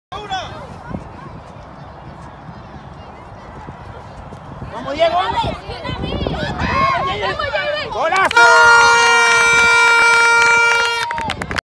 GOLAZO at Acoba Cup 2019
I contributed to my team victory with this goal ⚽️⚽️💥💥🏆🏆